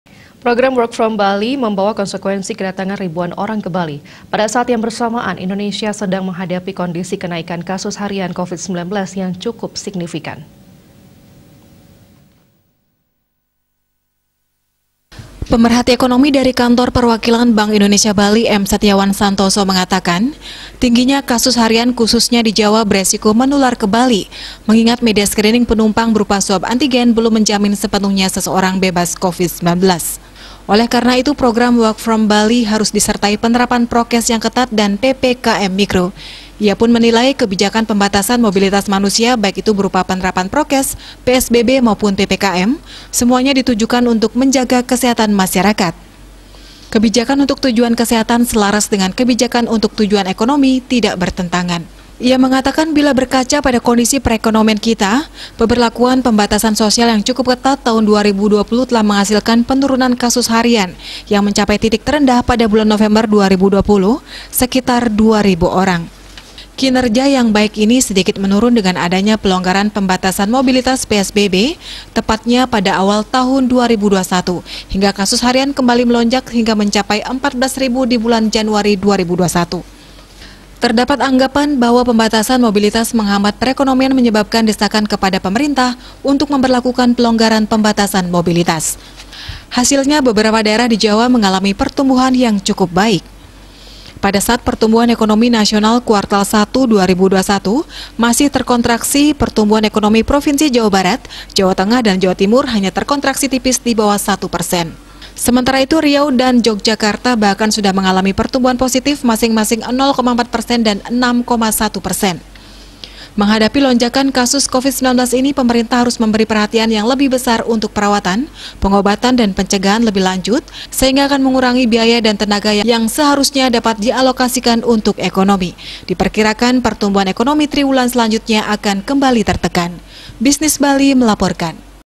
KORBANKAN KESEHATAN JUGA BEREFEK KE EKONOMI
PROGRAM WORK FROM BALI MEMBAWA KONSEKUENSI KEDATANGAN RIBUAN ORANG KE BALI. PADA SAAT YANG BERSAMAAN, INDONESIA SEDANG MENGHADAPI KONDISI KENAIKAN KASUS HARIAN COVID-19 CUKUP SIGNIFIKAN.